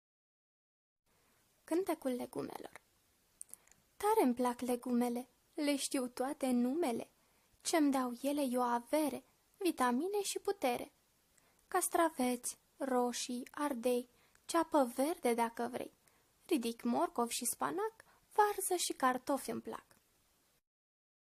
Cântecul legumelor- poezie
Cântecul legumelor

Tare-mi plac legumele,
Le ştiu toate numele.
Ce-mi dau ele-i o avere:
Vitamine şi putere!

Castraveţi, roşii, ardei,
Ceapă verde, dacă vrei,
Ridichi, morcovi şi spanac,
Varză şi cartofi – îmi plac!